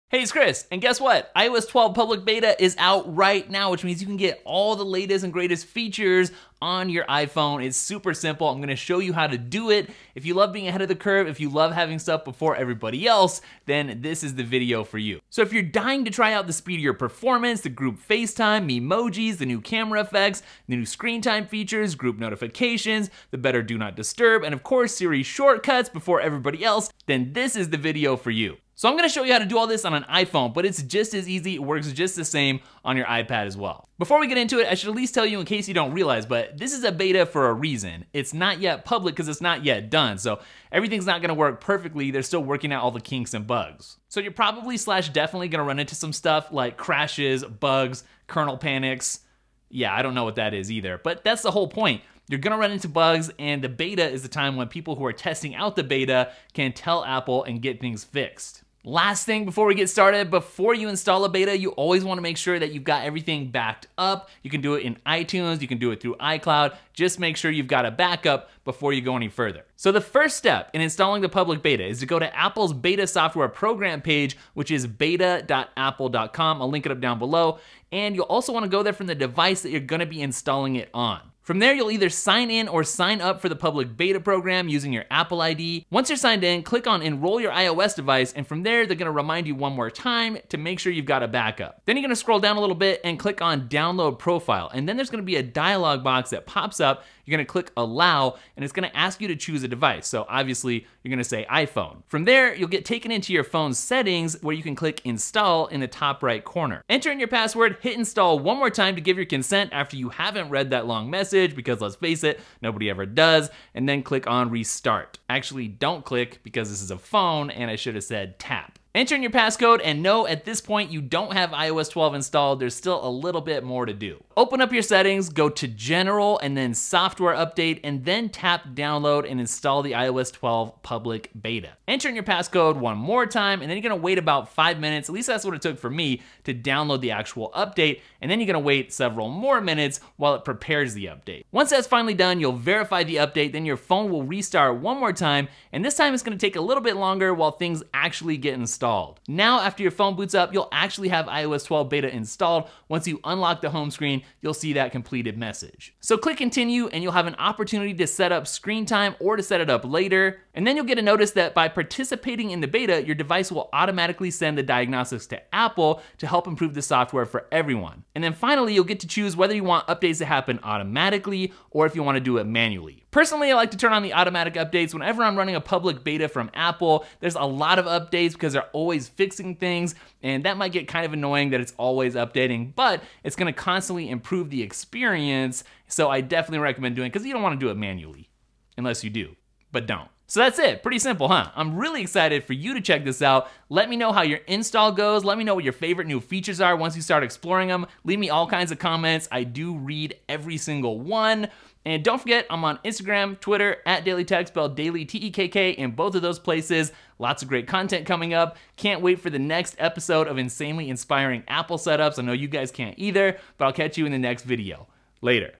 How To Install iOS 12 Public Beta (The Right Way)
I'll walk you through how to install the new iOS 12 public beta!!!
Studio gear

People ask about these a lot, so here:
- Swinging Sticks Sculpture
- Levitating Moon
- Digital Art Screens

Here’s an interview I did recently